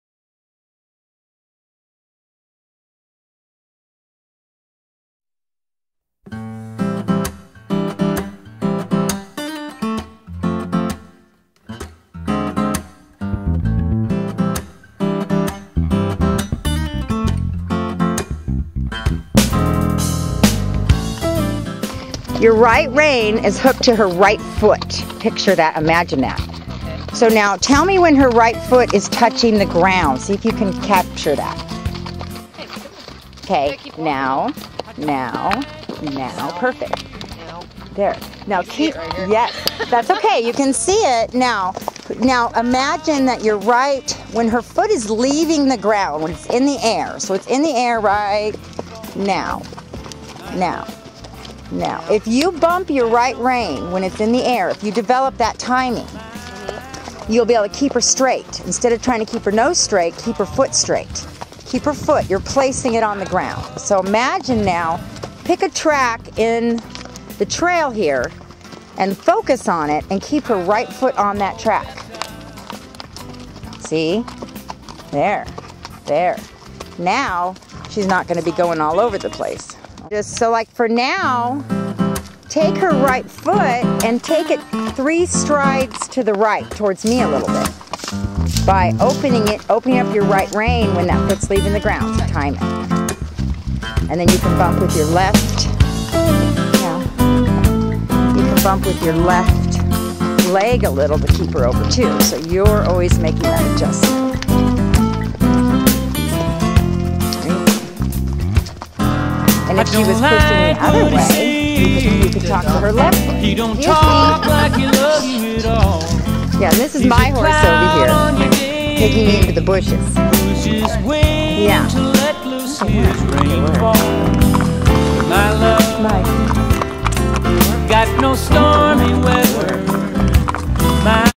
Lesson on the trail
"Straight from the horses mouth" Horse training. 3 min. that could change everything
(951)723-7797